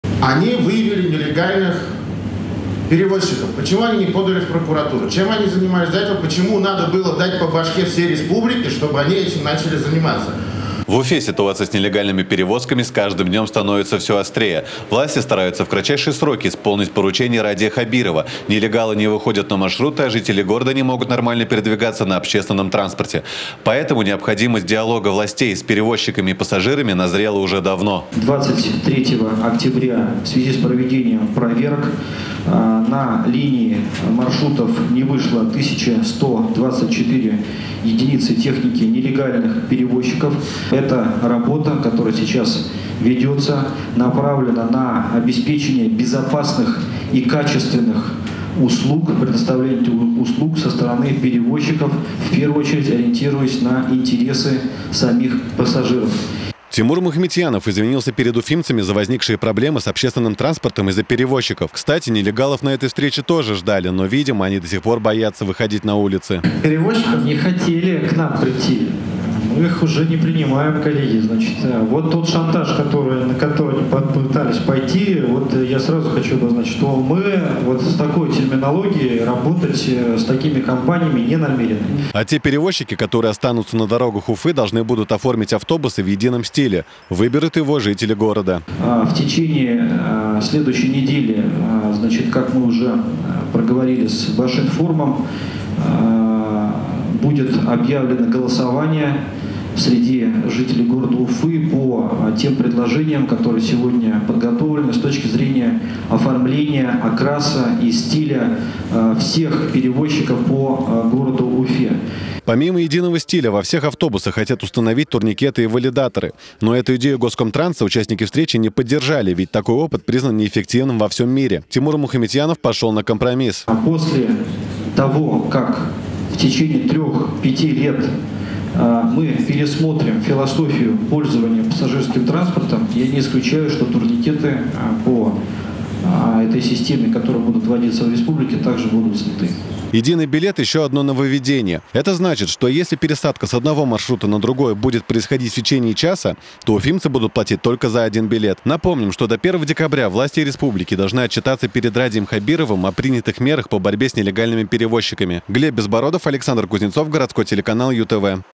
UTV. В Москве отказались от системы турникетов в автобусах. Теперь ее применят в Уфе
Единый стиль, единый билет и турникеты. Такие новшества ждут уфимский общественный транспорт.